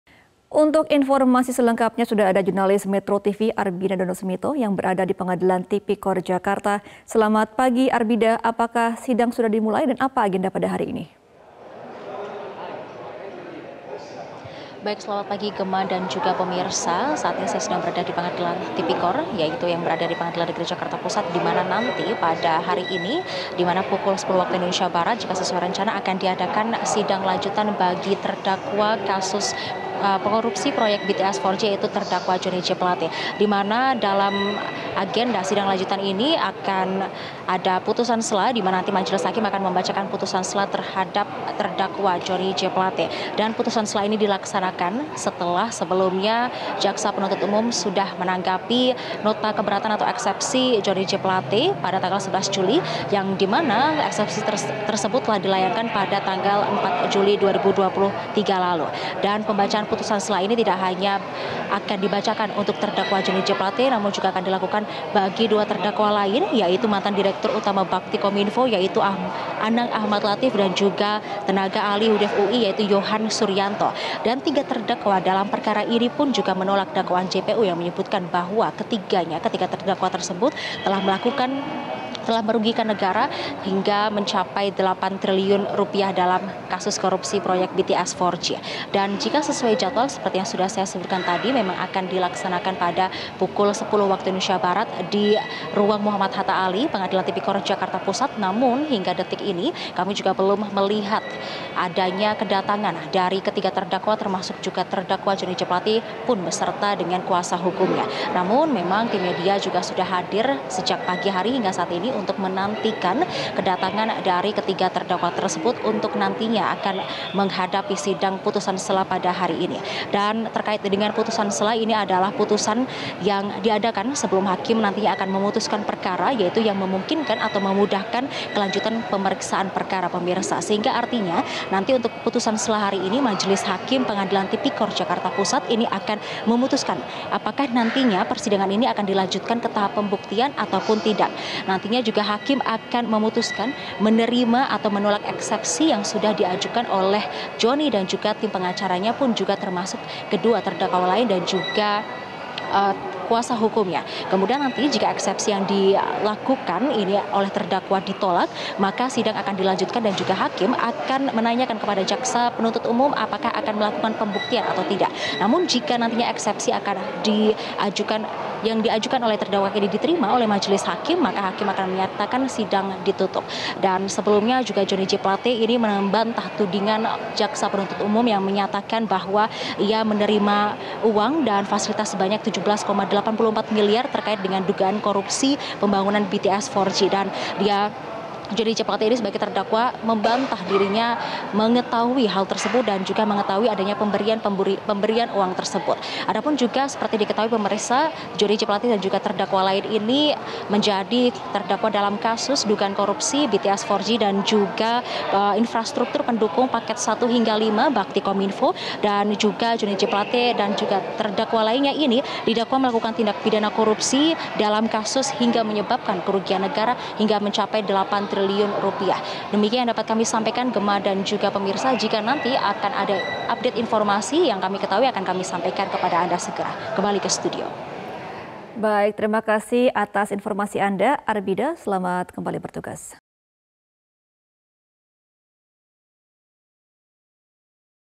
Eks Kominfo Jalani Sidang Putusan Sela di Tipikor Jakarta
MetroTV, Mantan Menteri Komunikasi dan Informatika atau Menkominfo, Johnny G Plate, dijadwalkan menjalani sidang putusan sela terkait kasus dugaan korupsi proyek pengadaan BTS 4G Kominfo di Pengadilan Tindak Pidana Korupsi atau Tipikor Jakarta pada Selasa (18/7/2023).